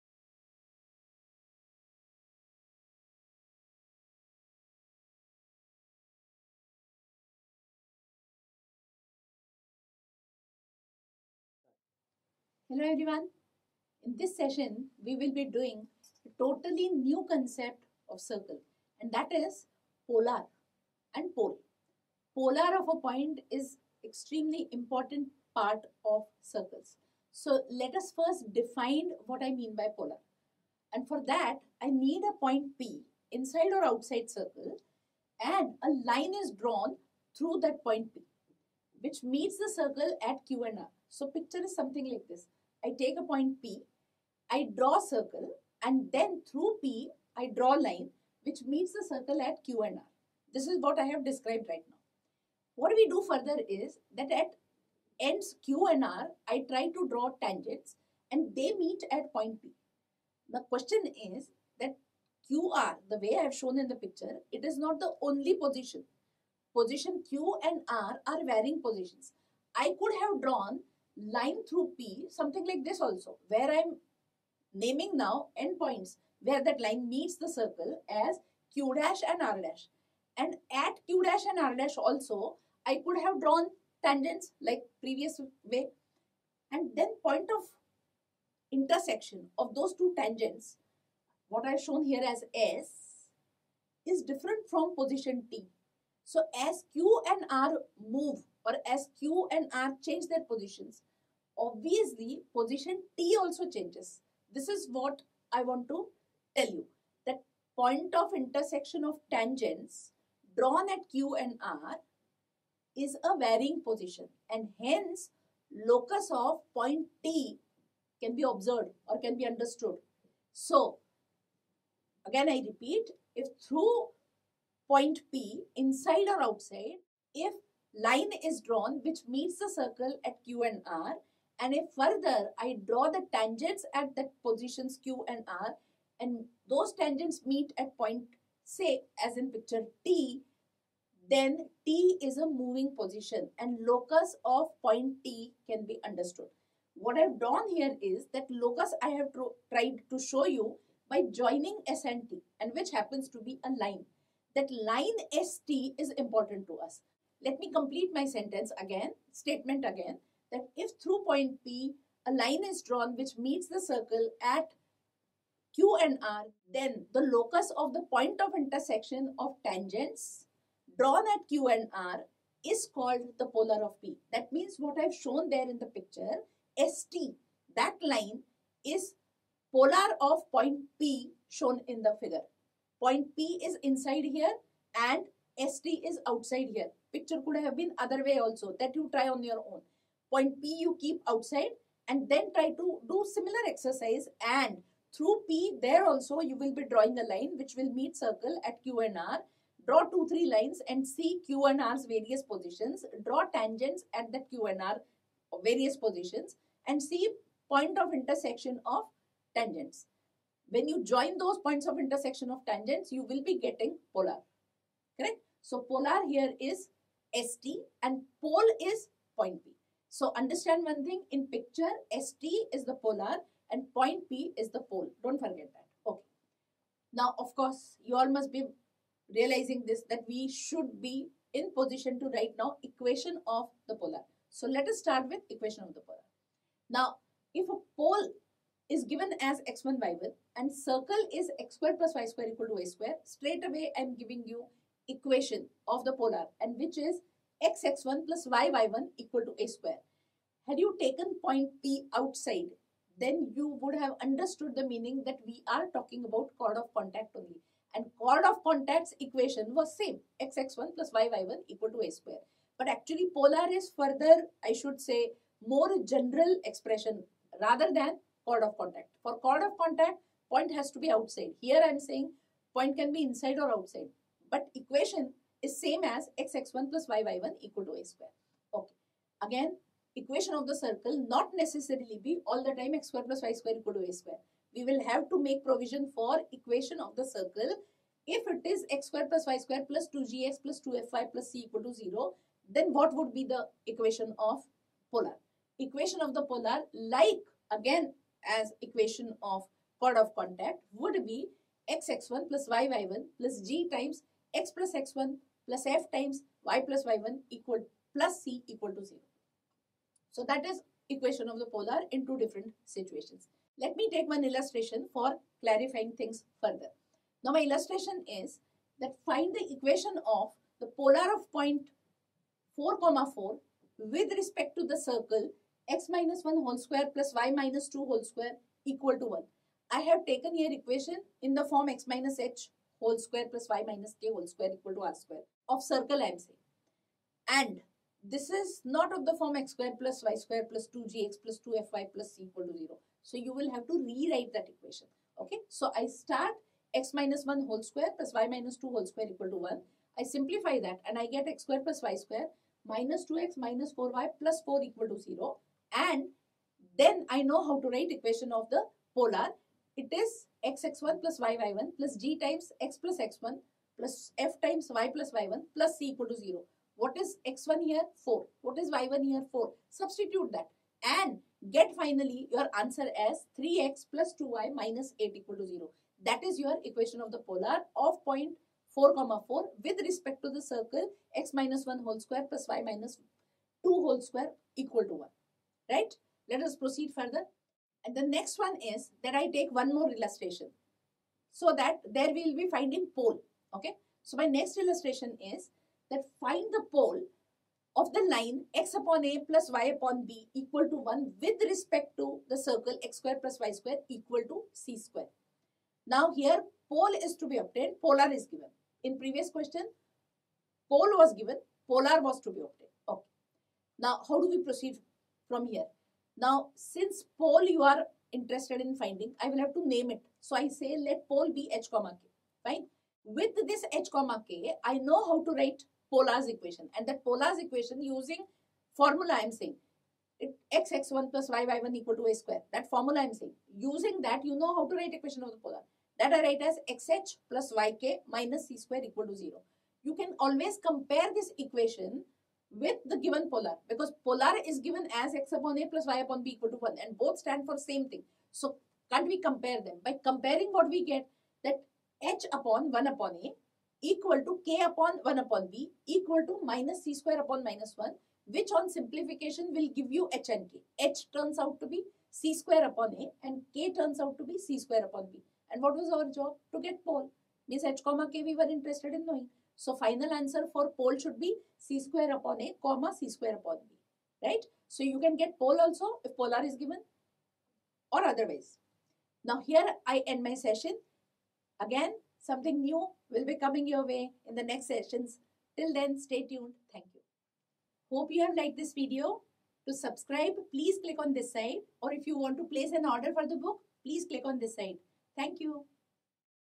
JEE - Mathematics - Circles - Pole and Polar
This video aims to aid the conceptual understanding of the following concepts:

1) Polar of a point with respect to a circle
2) pole corresponding to a polar
3) meaning of a polar
4) Equation of a polar with respect to a circle with centre at the origin
5) Equation of a polar with respect to a circle with centre not at the
    origin
6) Illustration on finding the equation of a polar with respect to a given
    circle
7) illustration on finding pole when polar with respect to a given circle
     is known

📕 JEE (Main) Challenger Maths Vol. - 1

📕 JEE (Main) Challenger Maths Vol. - 2

Check out the links below for more updates.